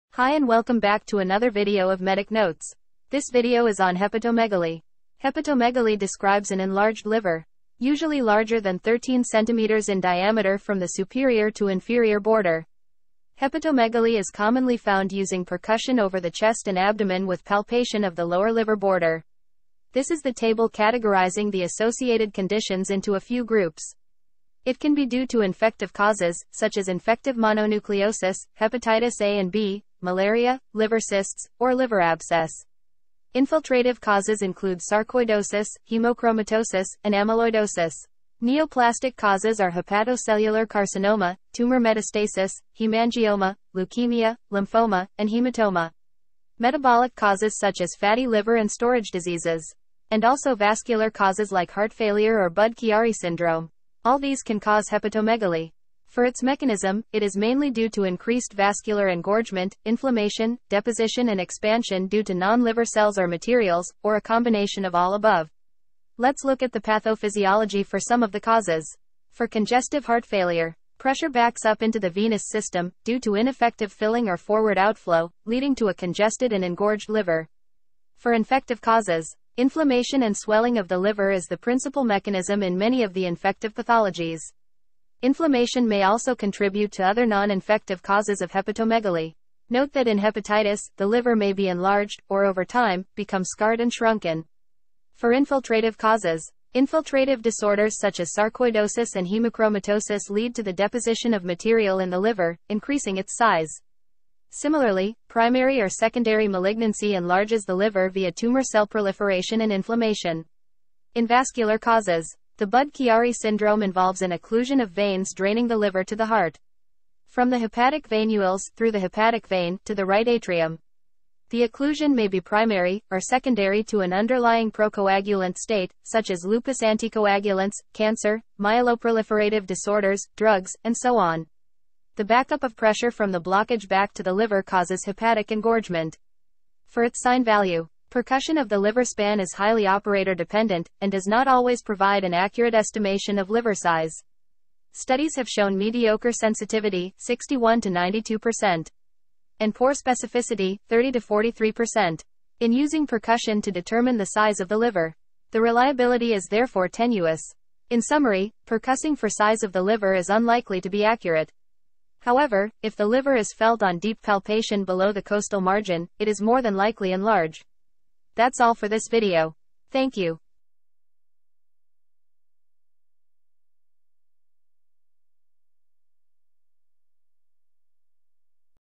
Hepatomegaly - causes, pathophysiology
An enlarged liver is one that's bigger than normal. The medical term is hepatomegaly. Rather than a disease, an enlarged liver is a sign of an underlying problem, such as liver disease, congestive heart failure or cancer.